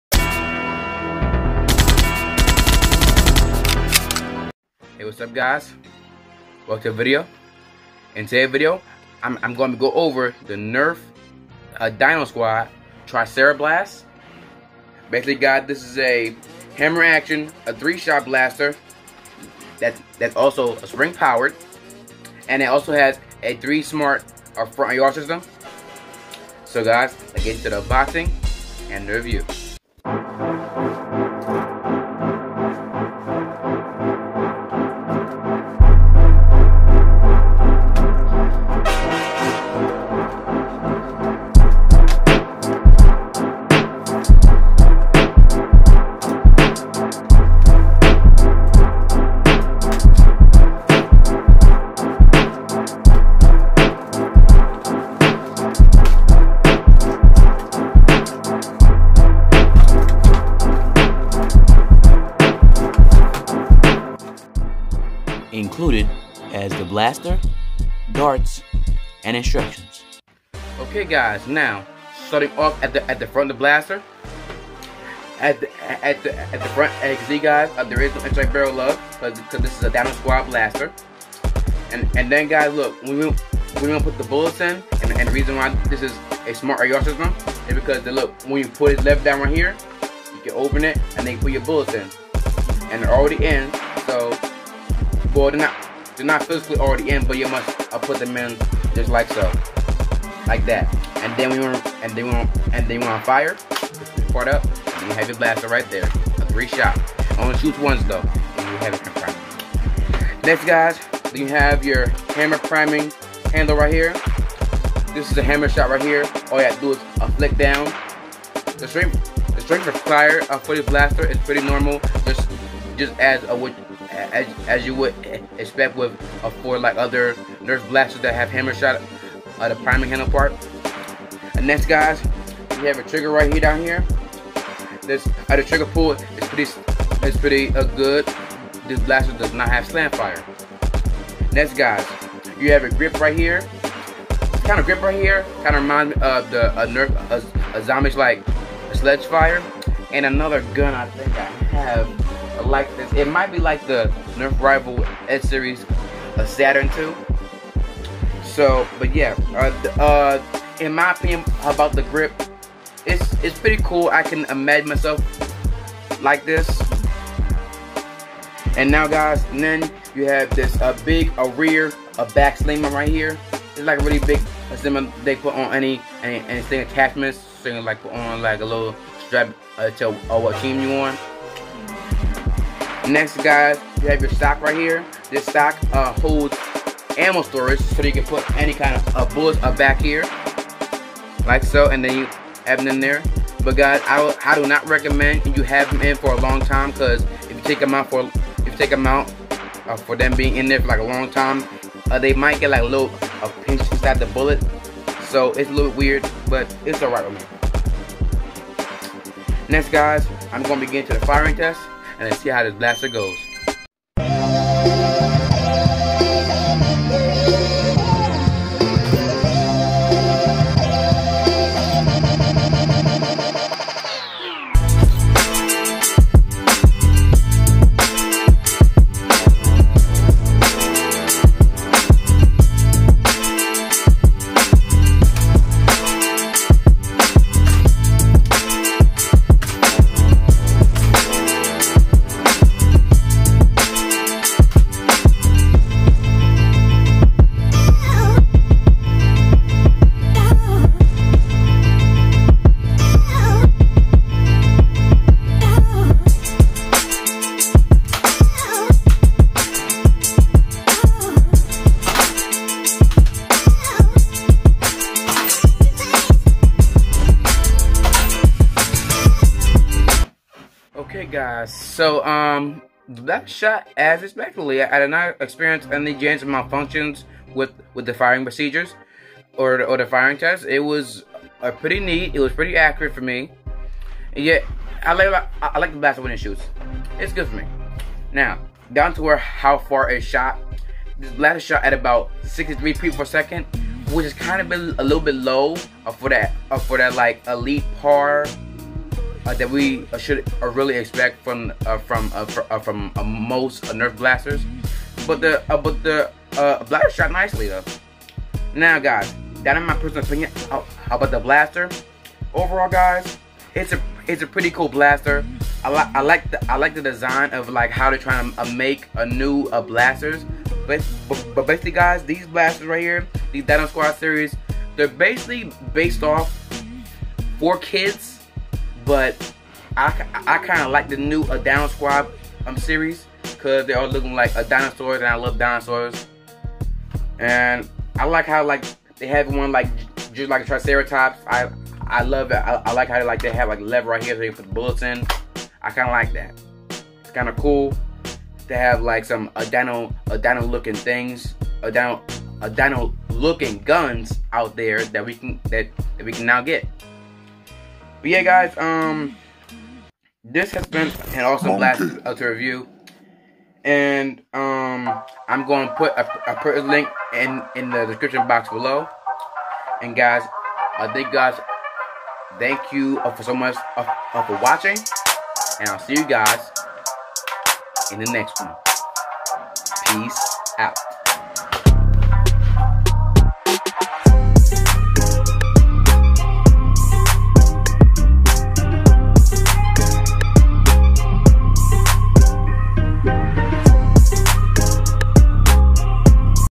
[Review] of the Nerf DinoSquad Tricera-Blast
Reviewing this awesome DinoSquad blaster that I got the other day which I think is awesome but the DinoSquad series is basically for kids because of the design.

Beginning: 0:05
Unboxing: 0:30
Review: 1:09
Firing Test: 3:35
Firing Analysis: 4:30
Personal Opinion: 5:10